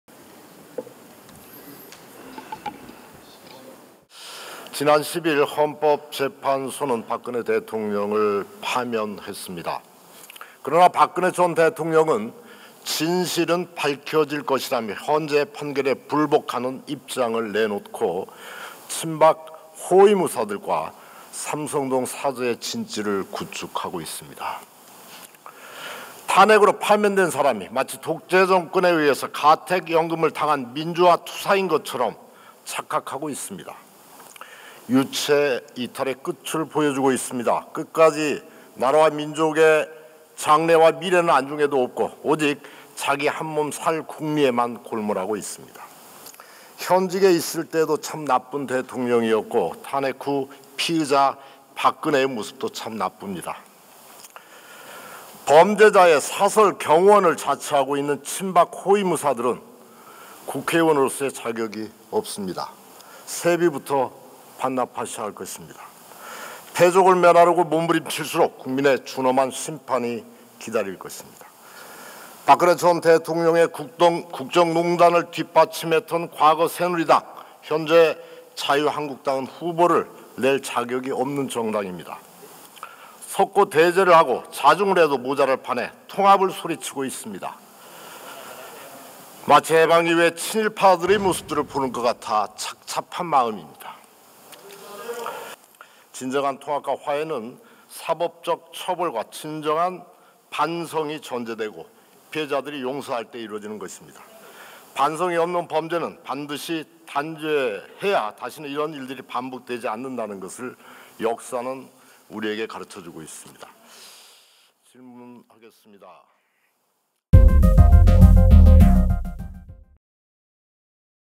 [현장] “친박 호위무사 의원들, 해방 후 친일파 같아”
박근혜 전 대통령과 친박 의원들을 향한 김광수 국민의당 의원의 묵직한 한마디